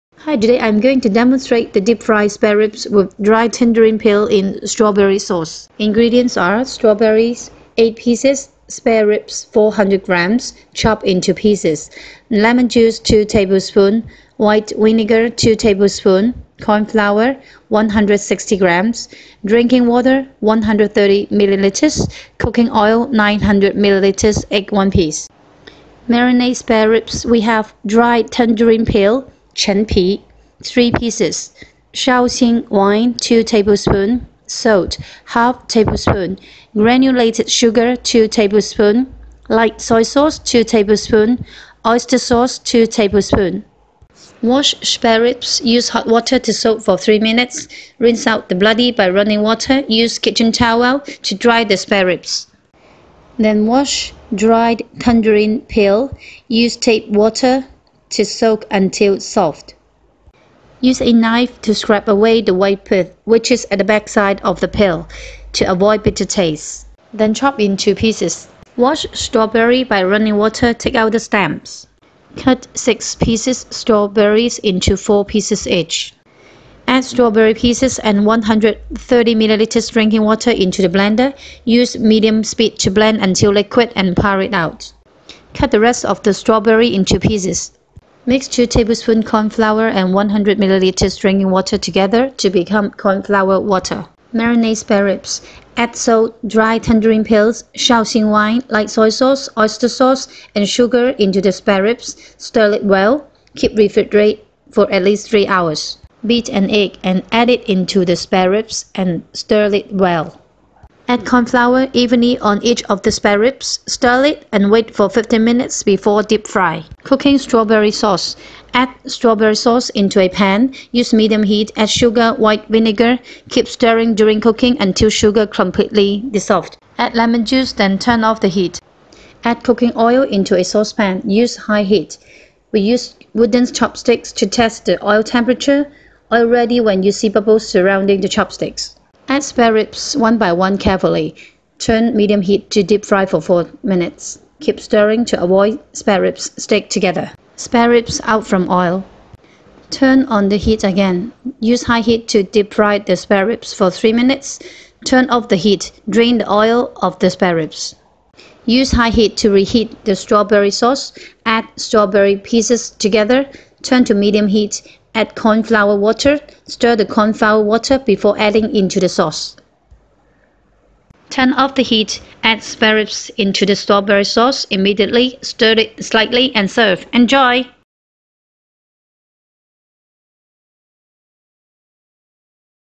Deep fried spare ribs with dried tangerine peel in strawberry sauce
Ingredients :
1.Strawberry 8 pcs
2.Spare ribs 400g (chop into pcs)
4.Lemon juice 2 tbsp
5.White vinegar 2 tbsp
6.Corn flour 160 grams
7.Drinking water 130 ml
8.Cooking oil 900 ml
9.Egg 1 pc

Marinate spareribs
1.Dried tangerine peel (Chen pi) 3 pcs
2.Shaoxing Wine 2 tbsp
3.Salt 1/2 tbsp
4.Granulated sugar 2 tbsp
5.Light soy sauce 2 tbsp
6.Oyster sauce 2 tbsp

Preparations and Steps:
1. Wash spare ribs, use hot water to soak for 3 mins, rinse out the bloody by running water; use kitchen towel to dry the spare ribs;
2. Wash dried tangerine peel, use tape water to soak until soft;
3. Use knife to scrape away the white pith which is at the back side of the peel; (to avoid bitter taste);
4. Wash strawberry by running water, take out the stems;
5. Cut 6 pcs strawberry into 4 pcs each;
6. Add strawberry pcs and 130 ml drinking water into the blender, use medium speed to blend until liquid and pour it out;
7. Cut the rest of strawberry into pcs ;
8. Mix 2 tbsp corn flour and 100 ml drinking water together to become corn flour water;

Marinate the spare ribs:
Add salt, dried tangerine peels, Shaoxing wine, light soy sauce, oyster sauce and sugar into the spare ribs, stir is well, keep refrigerate for at least 3 hours;

Cooking strawberrysauce
1. Add strawberry sauce into a pan, use medium heat, add sugar, white vinegar, keep stirring during cooking until sugar completely dissolved;
2.Add lemon sauce, then turn off the heat;

Deep fried spare ribs: (deep fried for 2 times)
1. Beat an egg and add it into the spare ribs and stir it well;
2.Add corn flour evenly on each of the spare ribs, stir it and wait for 15 mins before deep dry;
3. Add cooking oil into a saucepan, use high heat ; (use wooden chopsticks to test the oil temperature, oil ready when you see bubbles surrounding the chopsticks);
4.Add spare ribs one by one carefully, turn medium heat to deep fry for 4 mins (keep stirring to avoid spare ribs stick together;
5. Spare ribs out from oil;
6. Turn on the heat again, use high heat to deep fried the spare ribs for 3 mins, turn off the heat, drain the oil of the spare ribs;
7. Use high heat to re-heat the strawberry sauce, add strawberry pcs together, turn to medium heat, add corn flour water (stir the corn flour water before adding into the sauce)
8.Turn off the heat, add spare ribs into the strawberry sauce immediately, stir it slightly and serve;

Tips:
1. Shaoxing wine and dried tangerine peel can be available in most of the Chinese gourmet stores;
2. Increase the flavours of the spare ribs if marinate for overnight;
3. Hot water MUST NOT be use for making corn flour water.